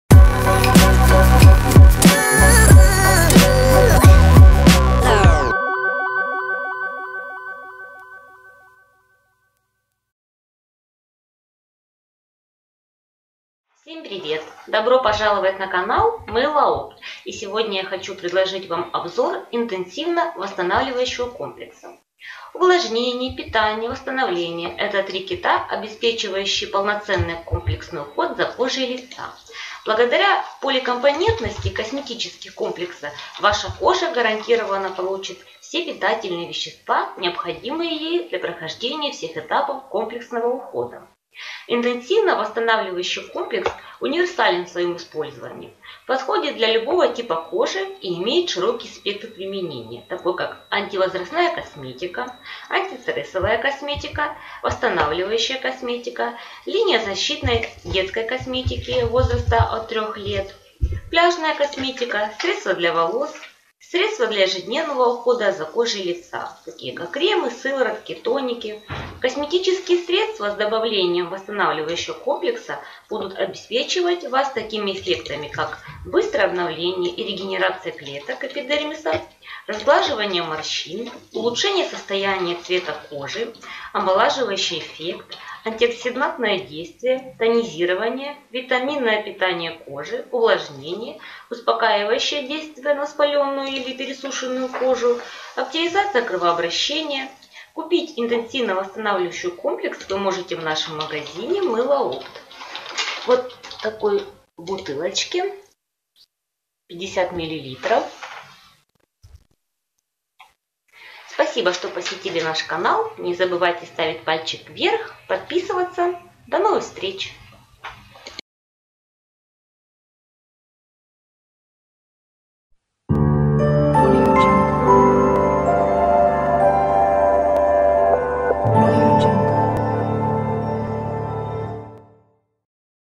Интенсивно Восстанавливающий комплекс// Все для красоты// От морщин
Видео обзор Интенсивно восстанавливающий комплекс​​ от магазина Мыло-опт